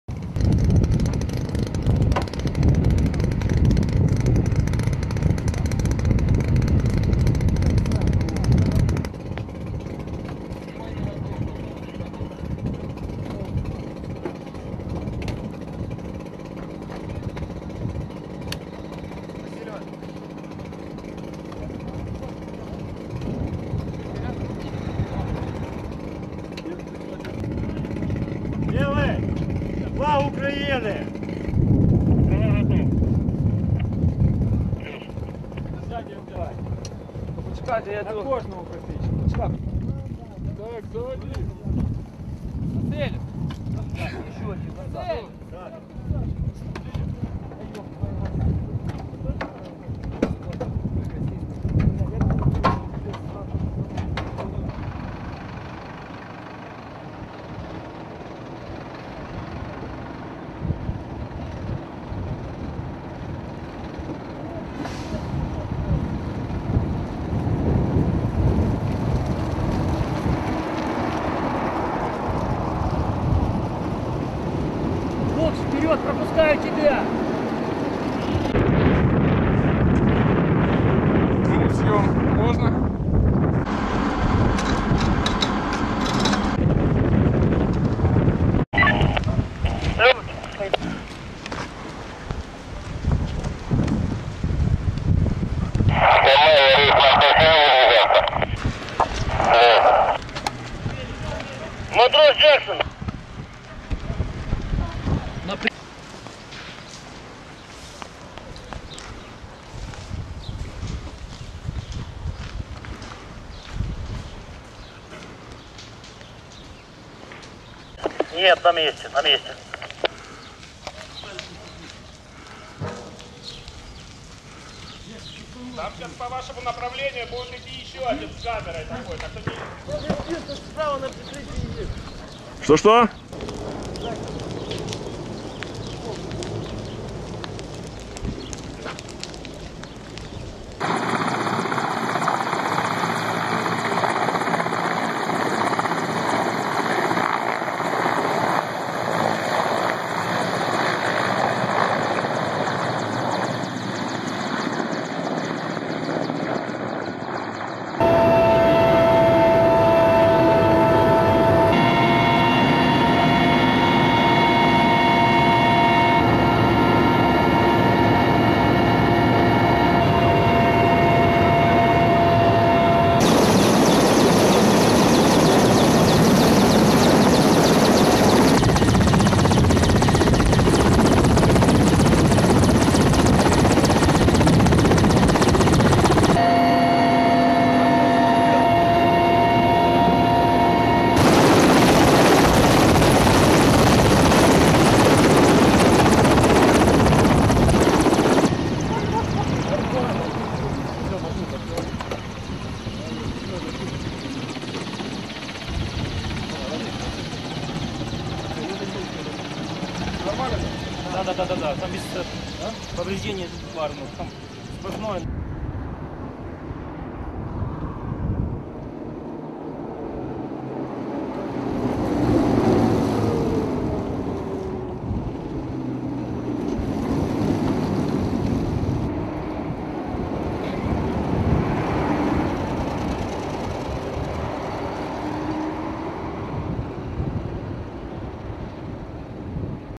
Antiterrorit Operation in Slovyansk: Video Report from Ukrainian Special Forces
This is a video report recorded by one of the special forces members who took part in the antiterrorist operation in Slovyans'k. This report gives a general overview on the attack, balockpost, wounded officer and the way he was transported to the medical facility.